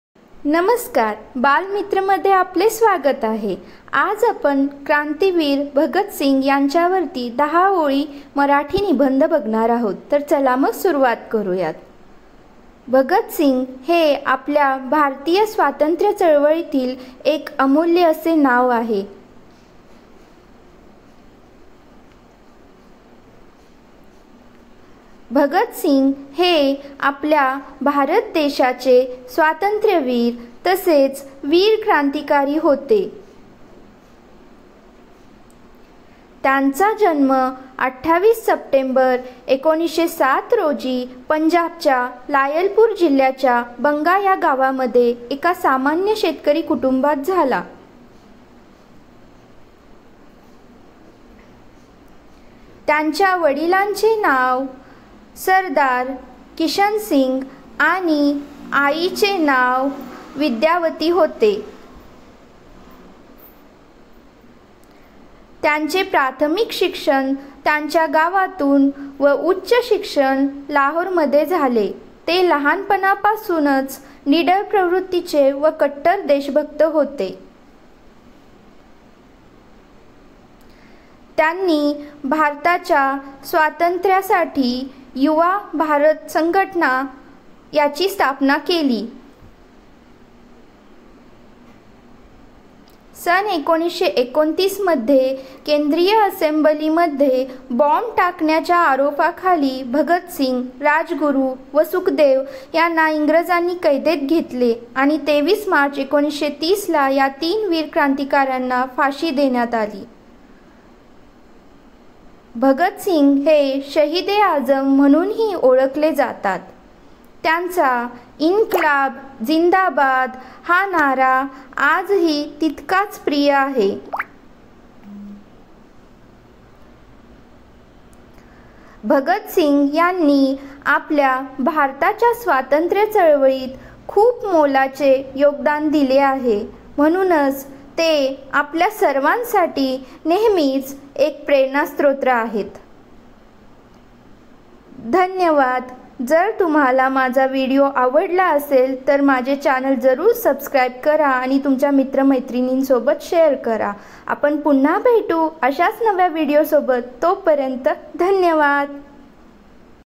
‘क्रांतिकारी भगतसिंग' 10 ओळी मराठी निबंध || 10 lines on Bhagatsing in marathi || Essay on Bhagatsing
हा व्हिडीओ क्रांतिवीर भगतसिंग यांच्या वरती दहा ओळी सुंदर असा निबंध प्रस्तुत करतो. हा विद्यार्थी तसेच शिक्षक वर्गासाठी उपयुक्त असा व्हिडिओ आहे. जर तुम्हाला माझ्या व्हिडिओ आवडला असेल तर चॅनल सबस्क्राईब करा आणि तुमच्या मित्र मैत्रिणींसोबत शेअर करा. धन्यवाद..!